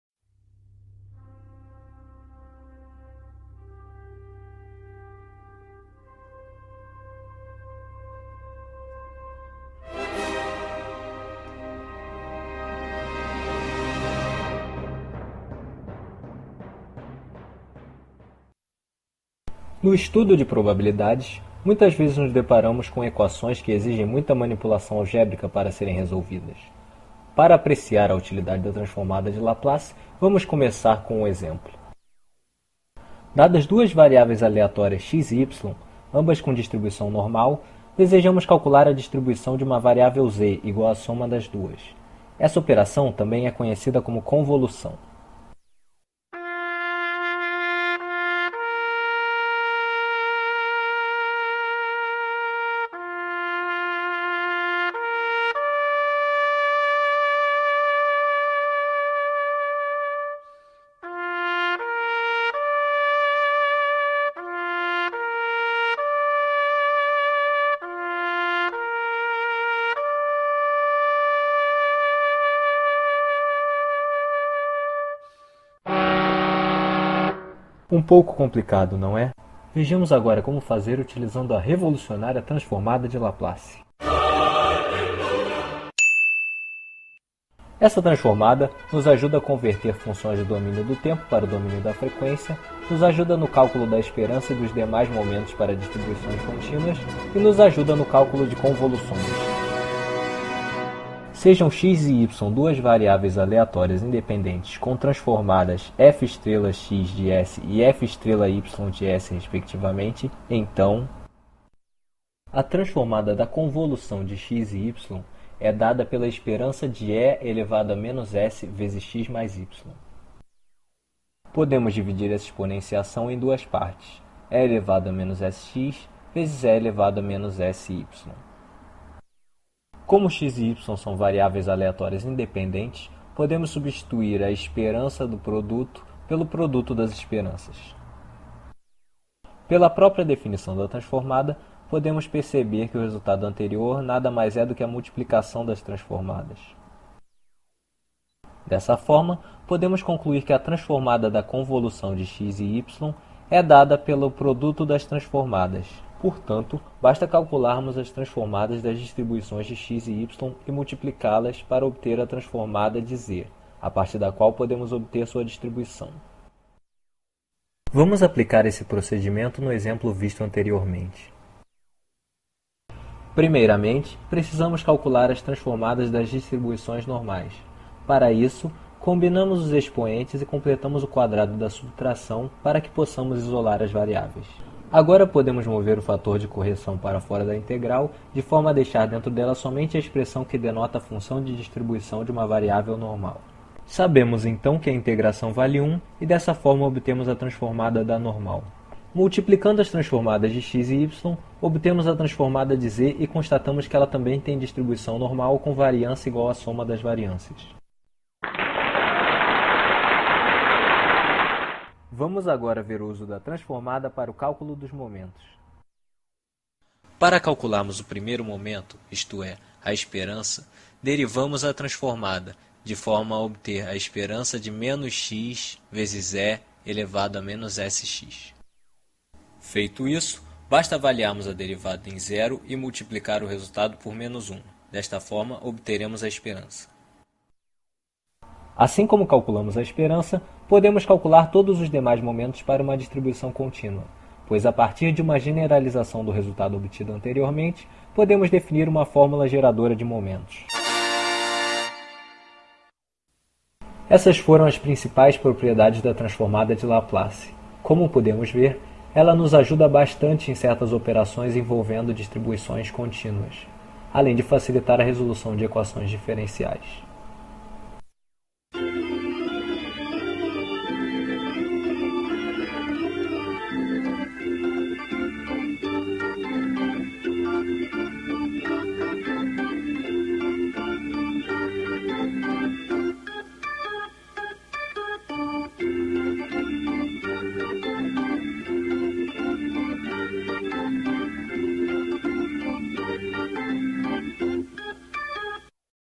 Transformada de Laplace
Transformada de Laplace para o cálculo de convoluções e momentos de variáveis aleatórias.